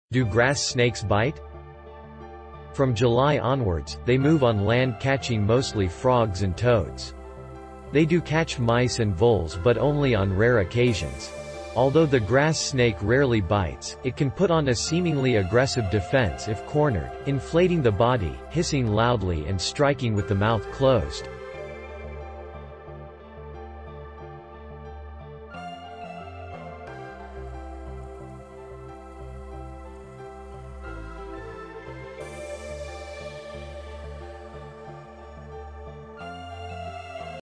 Do grass snakes bite?
From July onwards, they move on land catching mostly frogs and toads. They do catch mice and voles but only on rare occasions. Although the Grass Snake rarely bites, it can put on a seemingly aggressive defence if cornered, inflating the body, hissing loudly and striking with the mouth closed.

Reference:
Grass Snake | BTO - British Trust for Ornithology